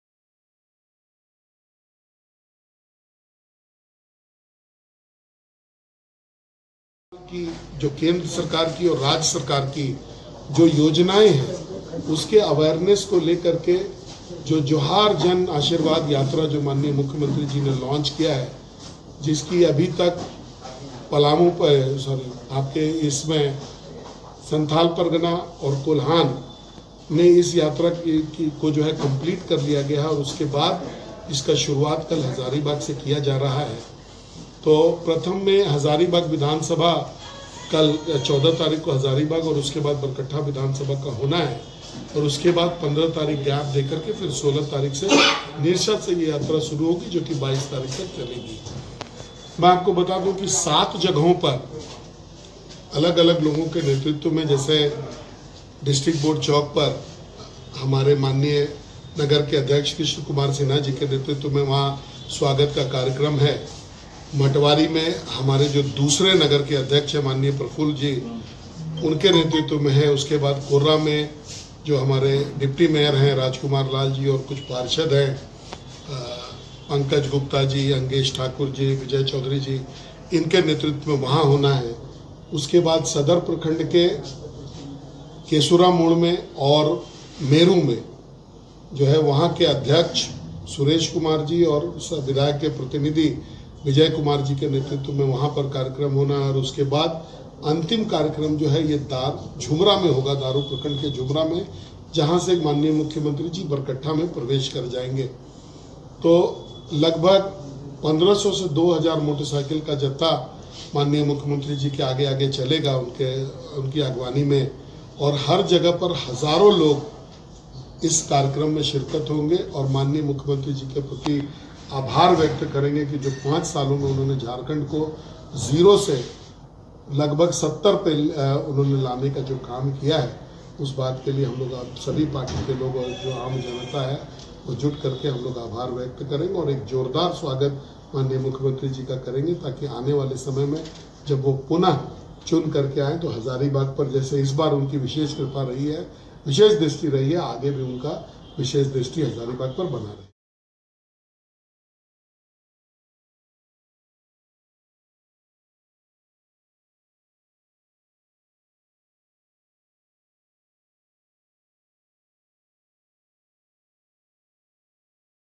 Hazaribagh से बीजेपी शुरू कर रही है जोहार जन आशीर्वाद यात्रा
हजारीबाग में भारतीय जनता पार्टी की झारखंड राज्य में विगत पांच वर्षों तक चली स्थिर सरकार की उपलब्धियों को जन- जन तक पंहुचाने के लिए शुरू किये गये जोहार जन आशीर्वाद यात्रा के क्रम में आगामी 14 अक्टूबर 2019, दिन- सोमवार को सूबे के मुख्यमंत्री रघुवर दास का आगमन हजारीबाग सदर विधानसभा क्षेत्र की धरती पर होने जा रहा है| इसकी जानकारी सदर विधायक मनीष जायसवाल ने अपने झंडा चौक स्थित विधायक कार्यालय परिसर में प्रेस- वार्ता कर दी...